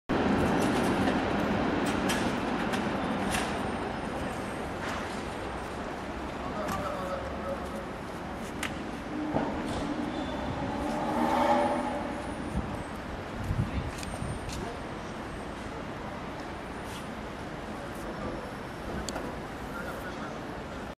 Fotograf niedaleko Urzędu Miejskiego we Wrocławiu
Za trzydzieści kilka złotych zrobisz od ręki zdjęcia potrzebne do różnych dokumentów np dowodu osobistego czy prawa jazdy.

Wydział Spraw Obywatelskich
ul. G. Zapolskiej 4, 50-032 Wrocław,